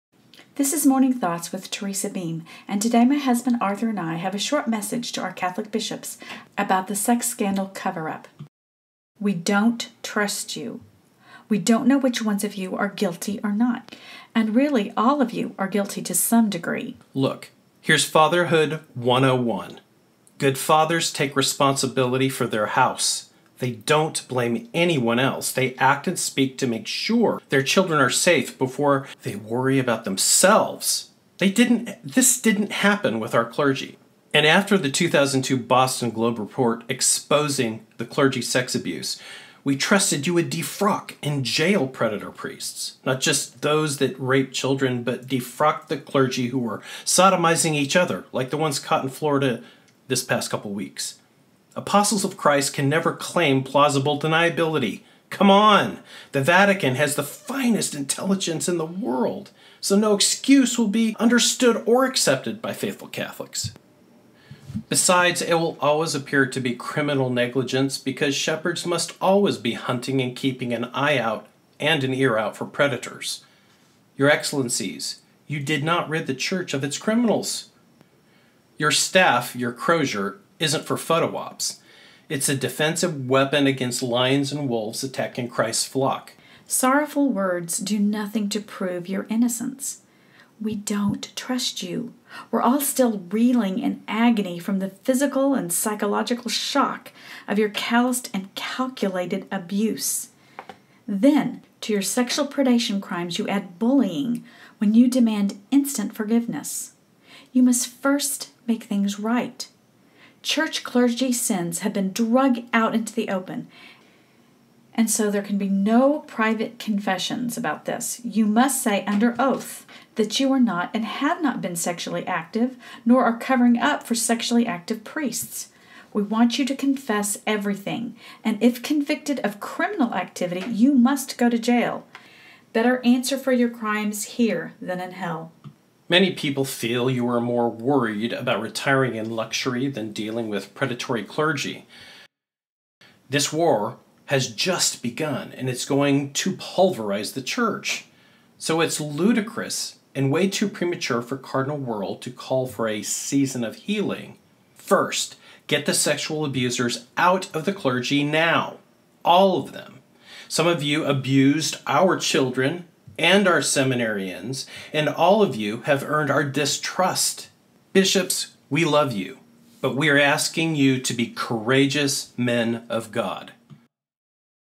BISHOPS WE DON'T TRUST YOU
A plea to Cardinal McCormick, Cardinal Wuerl, Cardinal Cupich, Cardinal Tobin, Cardinal Dolan, and all the U. S. Bishop and priests.